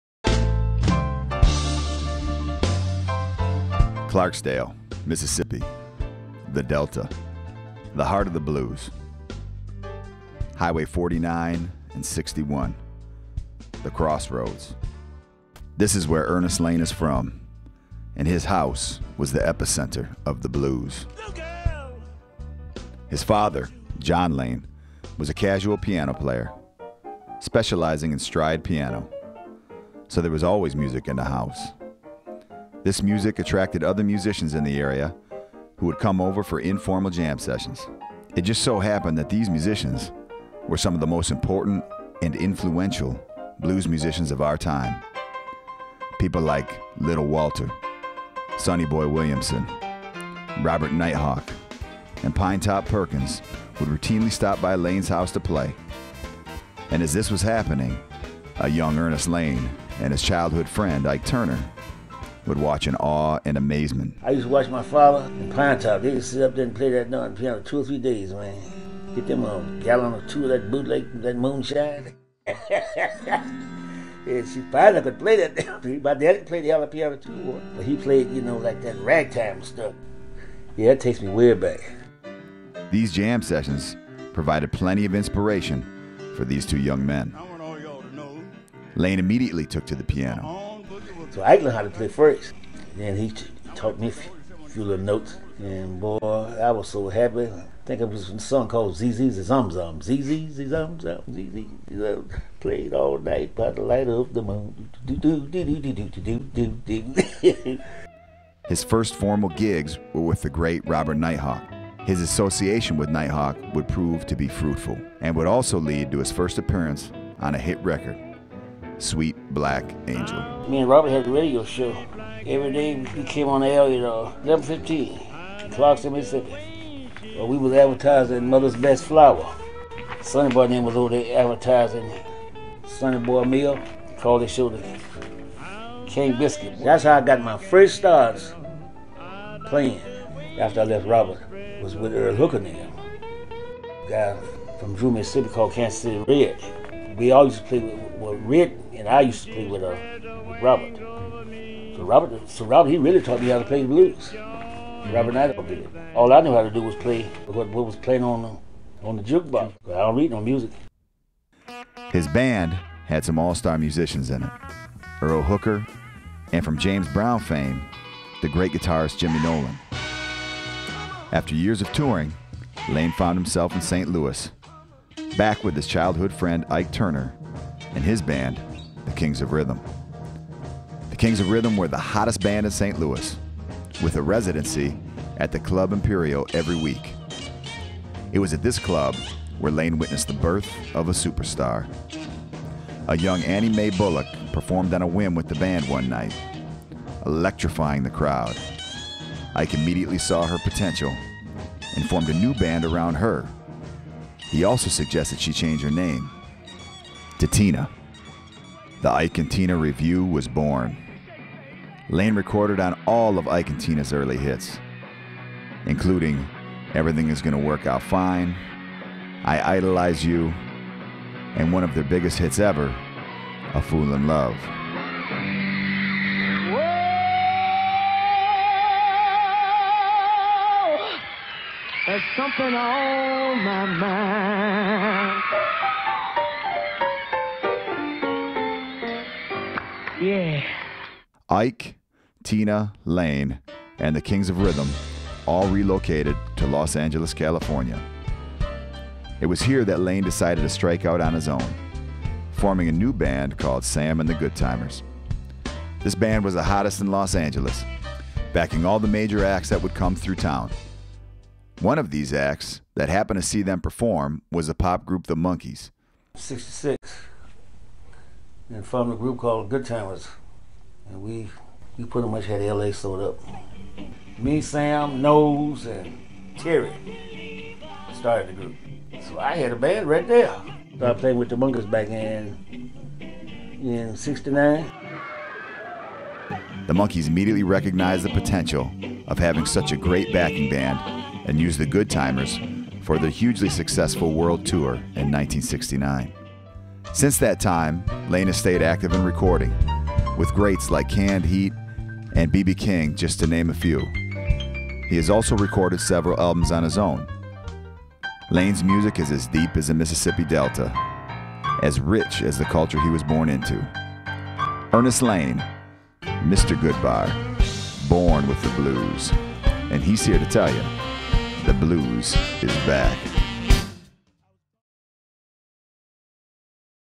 Ernest Lane Graphic Documentary
Graphic portrait of blues great Ernest Lane. Produced by Seth Blumberg, Design and animation by Matthew Giffin.